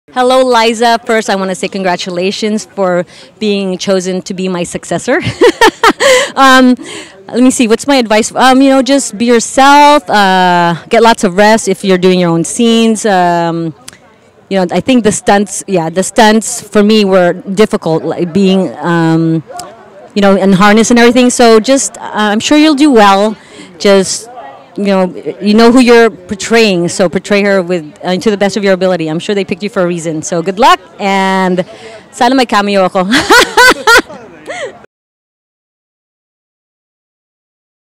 Former Darna star Anjanette Abayari congratulates Liza Sobaerano
After her acting stage debut in George Montero's "Living With Lola," Anjanette Abayari took a moment to congratulation newly announced actress to play Darna, Liza Soberano.

She also gives Liza some advice about playing Darna.

"Living With Lola" is currently playing at the Sacred Fools Theatre in Hollywood, CA.

"That's So Hollywood" opening title sequence by Matt Baguinon
Photos by Sthanlee B. Mirador